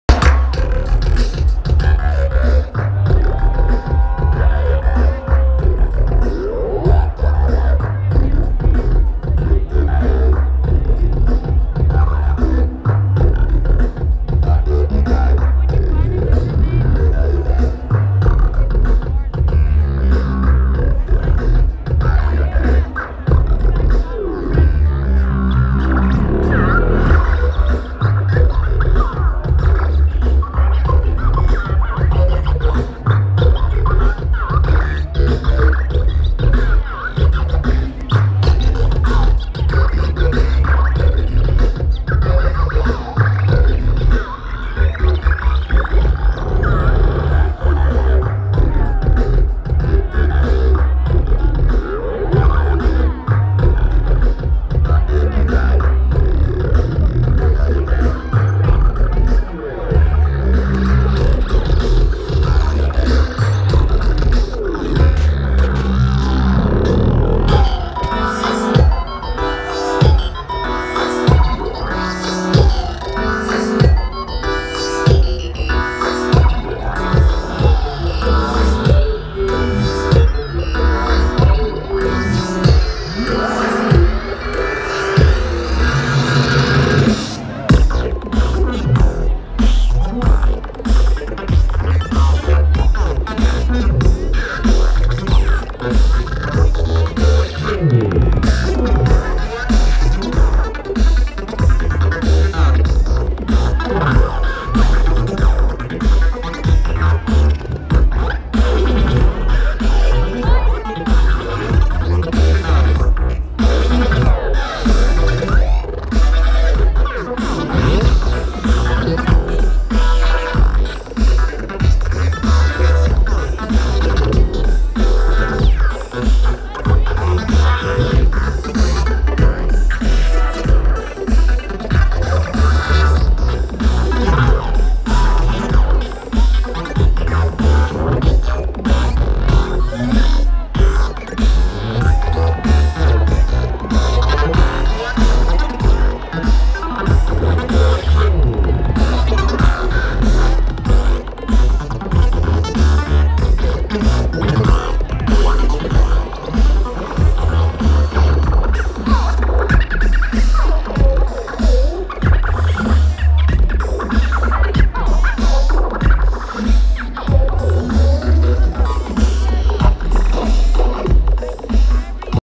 Tipper - Red Rocks 2015 - Uptempo Set
The debut of Tipper @ Red Rocks Amphitheatre - Morrison, Colorado
The first Red Rocks show hosted by Cervantes' Masterpiece Ballroom